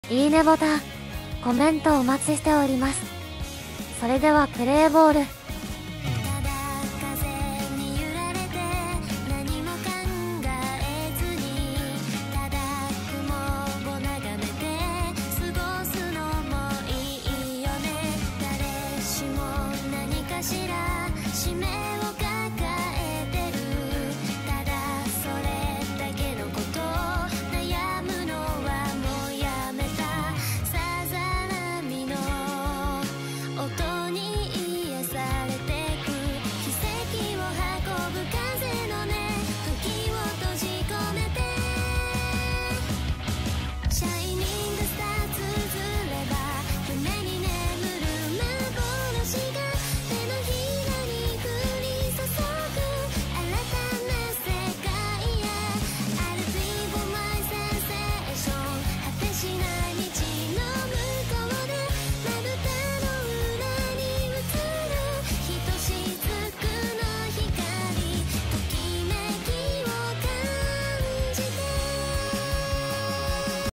PL学園高等学校OBベストナイン＆スタメンオーダー（桑田真澄/清原和博/松井稼頭央/新井宏昌/福留孝介）
PL学園高等学校OBのプロ野球選手でベストナイン＆スタメンオーダーを考えました。
◎皆さんの思うベストナインをコメント欄にお願いします。